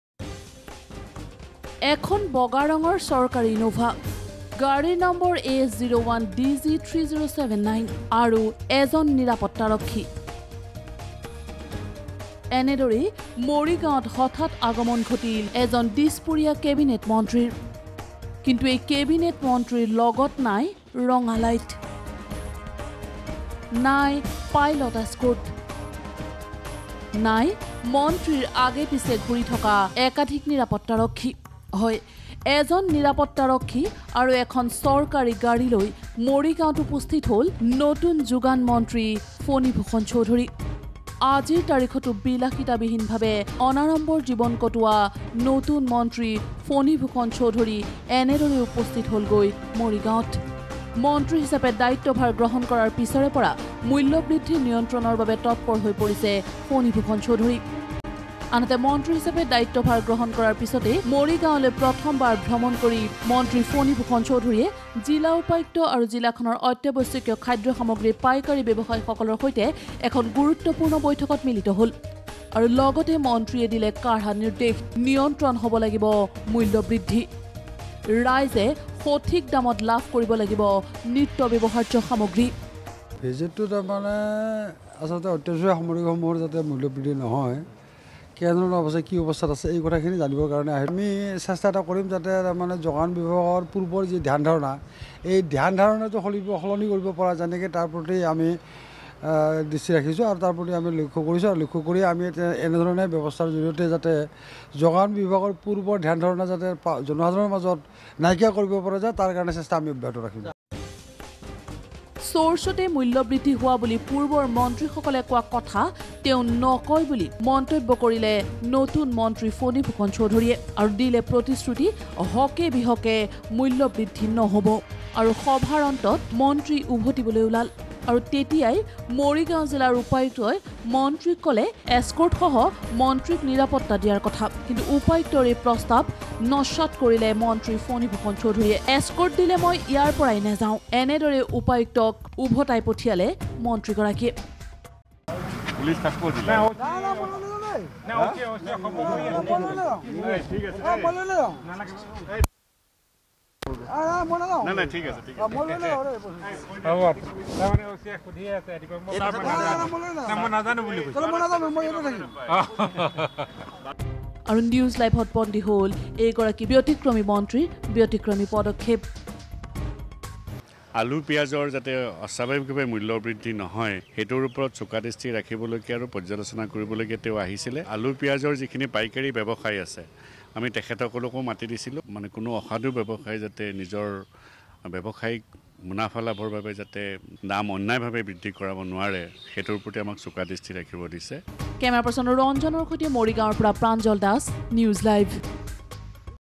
Minister Phani Bhushan Choudhury travels without police escort, no red beacon in car
ৰঙা লাইট আৰু এছকৰ্ট পৰিহাৰ কৰা দিছপুৰৰ এজন কেবিনেট মন্ত্ৰী। মাথোঁ এজন দেহৰক্ষী লৈ মৰিগাঁৱত উপস্থিত হৈ মূল্যবৃদ্ধি নিয়ন্ত্ৰণৰ ঘোষণা যোগান মন্ত্ৰী ফণীভূষণ চৌধুৰীৰ।